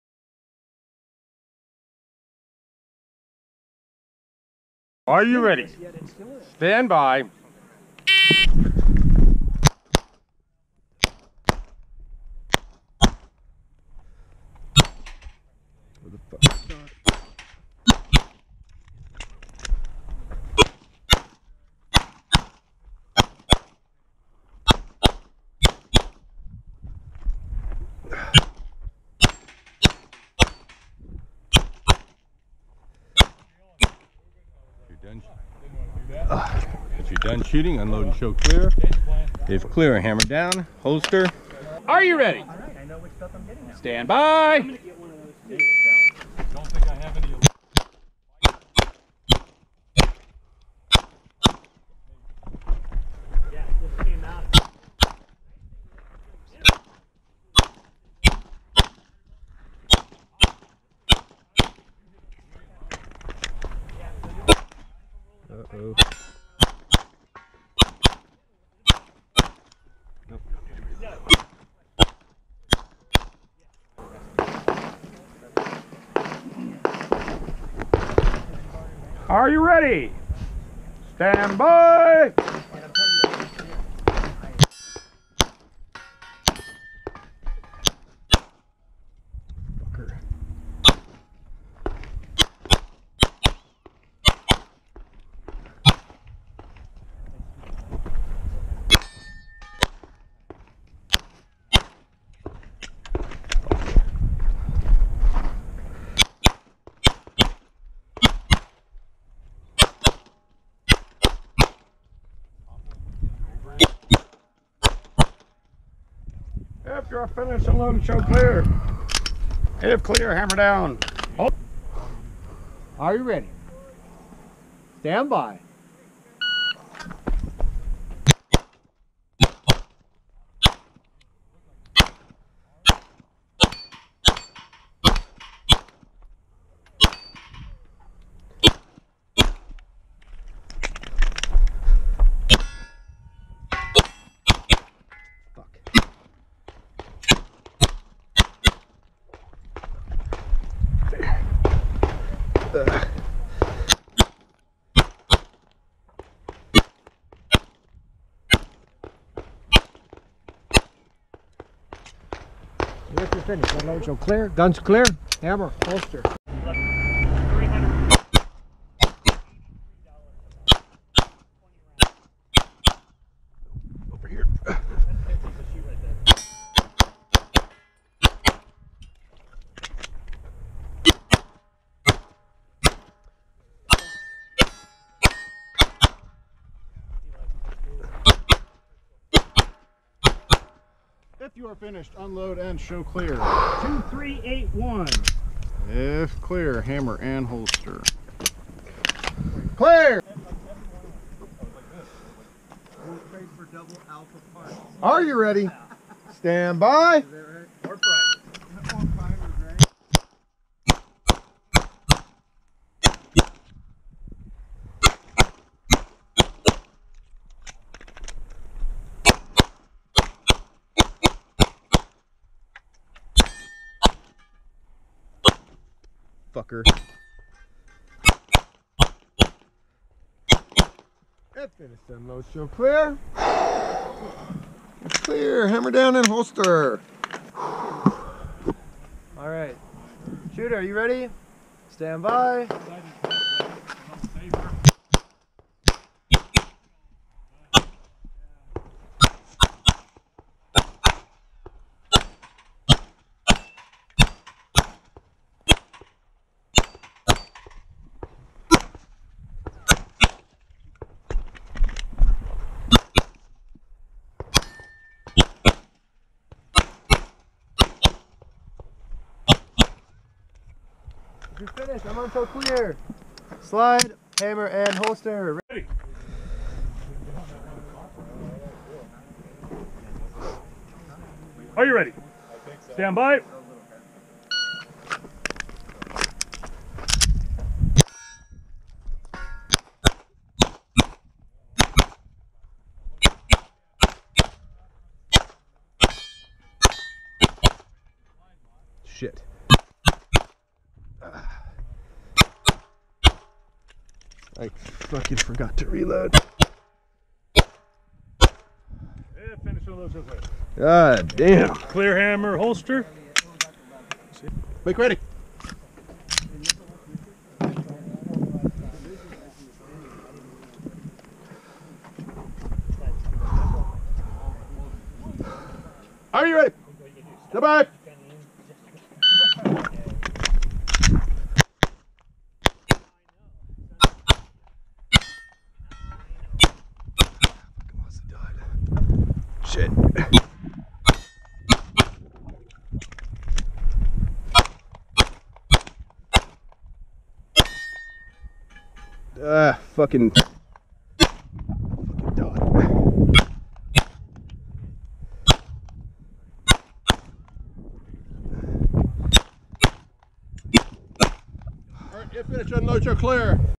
LDF Sectional USPSA Match Sept 2021
Using a factory Glock 17 MOS with C&H plate for RMR 3.25

Forgive the terrible editing.  This is the first video I've really tried to edit and it did not turn out that awesome.